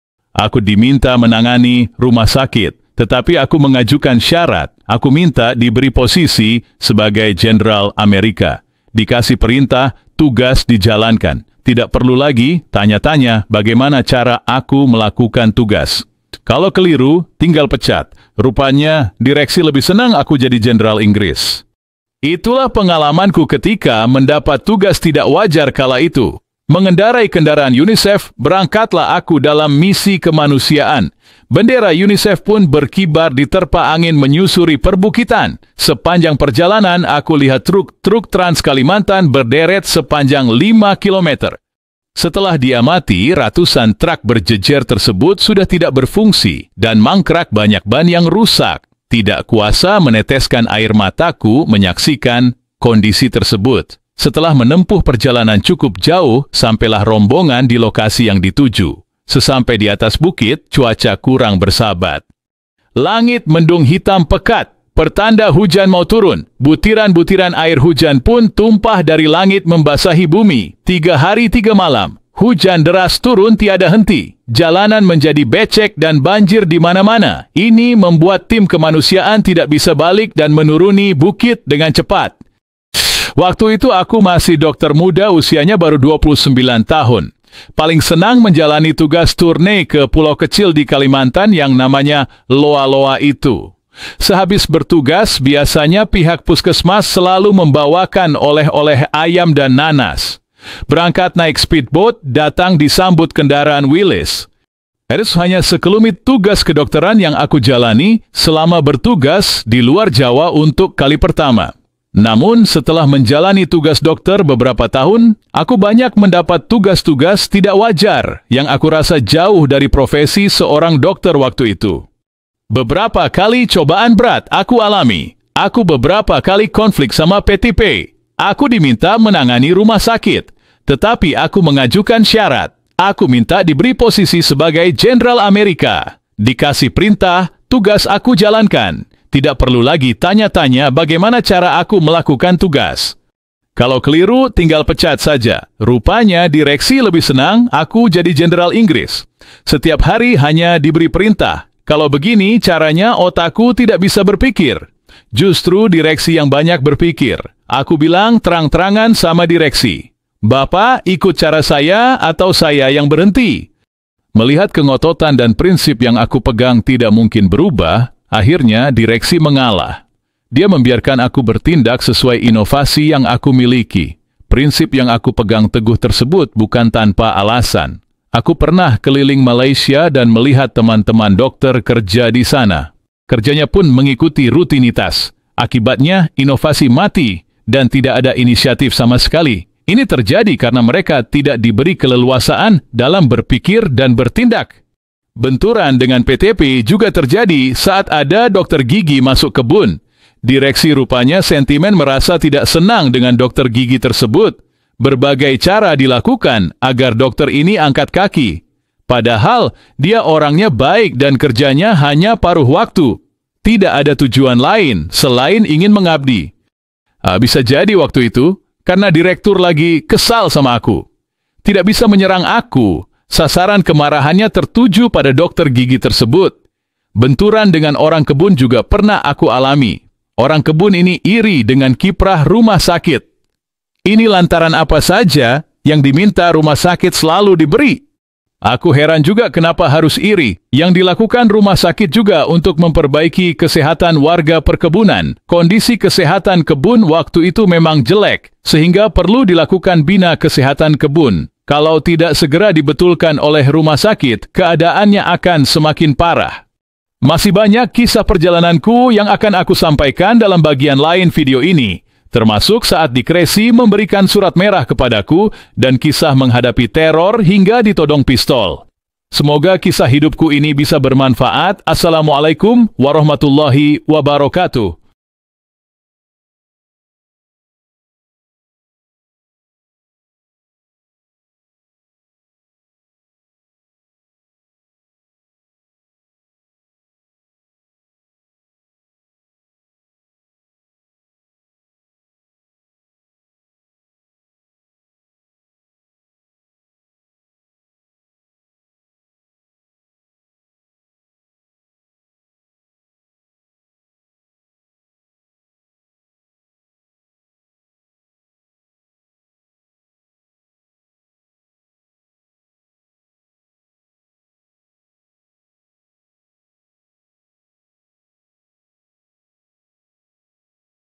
Inginnya Menjadi Jenderal Amerika Bukan Jenderal Inggris//Kisah Si Moai (Part4)
Inginnya Menjadi Jenderal Amerika Bukan Jenderal Inggris
Kisah Si Moai Ingin Jadi Dokter (Part4)
Musytahar Umar Thalib yang akarab disapa Si Moai ini sempat diminta menangani rumah sakit yang kondisinya parah. Tetapi Musytahar sempat mengajukan syarat. Musytahar minta diberi posisi sebagai `Jenderal Amerika`. Dikasih perintah, tugas dijalankan. Tidak perlu lagi tanya-tanya bagaimana cara Musytahar melakukan tugas. Kalau keliru, tinggal pecat saja. Rupanya direksi lebih senang jika Musytahar jadi `Jenderal Inggris`.

Itulah pengalaman Musytahar ketIka mendapat tugas tidak wajar kala itu. Kisahnya, saat itu dimulai saat Musytahar mengendarai kendaraan Unicef, berangkat dalam misi kemanusiaan. Bendera Unicef pun berkibar diterpa angin menyusuri perbukitan. Sepanjang perjalanan, Musytahar melihat truk- truk trans Kalimantan berderet sepanjang lima kilometer.
Setelah diamati, ratusan truk yang berjejer tersebut sudah tidak berfungsi dan mangkrak, banyak ban yang rusak. Tidak kuasa Musytahar meneteskan air mata menyaksikan kondisi tersebut.
Setelah menempuh perjalanan cukup jauh, sampailah rombongan di lokasi yang dituju. Sesampai di atas bukit, cuaca kurang  bersahabat.
Langit mendung hitam pekat. Pertanda hujan mau turun. Benar saja. Tidak lama kemudian, butiran-butiran air hujan tumpah dari langit membasahi bumi.
Tiga hari tiga malam, hujan deras turun tiada henti. Jalanan menjadi becek dan banjir di mana-mana. Tentu saja ini membuat tim kemanusiaan tidak bisa balik dan menuruni bukit dalam waktu cepat.

Waktu itu, Musytahar masih dokter muda yang usianya baru 29 tahun. Paling senang menjalani tugas tourney ke pulau-pula kecil di Kalimantan yang namanya Loa-Loa itu. Sehabis bertugas, biasanya pihak puskesmas selalu membawakan oleh-oleh ayam dan nanas. Berangkat naik speed boat, datang disambut kendaraan Willis
Itu hanya sekelumit tugas kedokteran yang Musytahar jalani selama bertugas di luar Jawa untuk kali pertama. Namun, setelah menjalani tugas dokter beberapa tahun, Musytahar banyak mendapat tugas-tugas tidak wajar yang aku rasa jauh dari profesi seorang dokter waktu itu.
Beberapa kali cobaan berat Musytahar alami. Musytahar beberapa kali konflik sama PTP. Musytahar diminta menangani rumah sakit. Tetapi Musytahar mengajukan syarat. Musytahar minta diberi posisi sebagai Jenderal Amerika. Dikasih perintah, tugas dijalankan. Tidak perlu lagi tanya-tanya bagaimana cara Musytahar melakukan tugas.
 Kalau keliru, tinggal pecat saja. Rupanya direksi lebih senang Musytahar jadi `Jenderal Inggris`.  Setiap hari hanya diberi perintah. Kalau begini caranya, Musytahar merasa dirinya tidak bisa berpikir. Justru direksi yang banyak berpikir.
Musytahar kala itu bilang terang-terangan sama direksi. “Bapak ikut cara saya atau saya yang berhenti?, ujar Musytahar waktu itu..

Melihat kengototan dan prinsip yang Musytahar pegang tidak mungkin berubah, akhirnya direksi mengalah. Dia membiarkan Musytahar bertindak sesuai inovasi yang dimiliki. Prinsip yang Musytahar pegang teguh tersebut bukan tanpa alasan. Musytahar pernah keliling Malaysia dan melihat teman-teman dokter kerja di sana.
Teman-teman Musytahar memang kebanyakan kerja di Malaysia. Musytahar melihat, hidupnya begitu teratur, kerjanya pun mengikuti rutinitas. Akibatnya, inovasi mati dan tidak ada inisiatif sama sekali. Ini terjadi karena mereka tidak diberi keleluasaan dalam berpikir dan bertindak.
Benturan Musytahar dengan PTP juga terjadi saat ada dokter gigi masuk kebun. Direksi rupanya sentimen dan merasa tidak senang dengan dokter gigi tersebut. Berbagai cara dilakukan agar dokter ini angkat kaki. Padahal, dia orangnya baik dan kerjanya hanya paruh waktu. Tidak ada tujuan lain, selain ingin mengabdi untuk masyarakat.

Bisa jadi waktu itu, karena direktur lagi kesal sama Musytahar. Akhirnya tidak bisa menyerang Musytahar, maka sasarannya kemarahannya tertuju pada dokter gigi tersebut.
Benturan dengan orang kebun juga pernah Musytahar alami. Orang kebun ini iri dengan kiprah rumah sakit. Ini lantaran apa saja yang diminta rumah sakit selalu diberi oleh PTP.

Minta laboratorium dikasih. Meski lama-lama, PTP tidak kuat juga saat ada permintaan peralatan medis lainnya. Akhirnya, peralatan laboratorium ukuran besar-besar dari universitas Airlangga Surabaya yang berlemari-lemari itu yang dipakai. Saat mendapatkan fasilitas laboratoriumm, orang kebun tersebut tambah iri dan lebih kaget lagi.
Musytahar heran juga kenapa harus iri. Yang dilakukan rumah sakit juga untuk memperbaiki Kesehatan warga Perkebunan. Kondisi Kesehatan kebun waktu itu memang jelek. Sehingga perlu dilakukan bina Kesehatan kebun. Kalau tidak segera dibetulkan oleh rumah sakit, keadaaannya akan semakin parah.

Masih banyak kisah perjalanan Musytahar yang akan disampaikan dalam bagian lain video ini. Termasuk saat dikresi memberikan surat merah kepada Musytahar dan kisah  menghadapi terror hingga ditodong pistol. Semoga kisah hidup Musytahar ini bisa bermanfaat,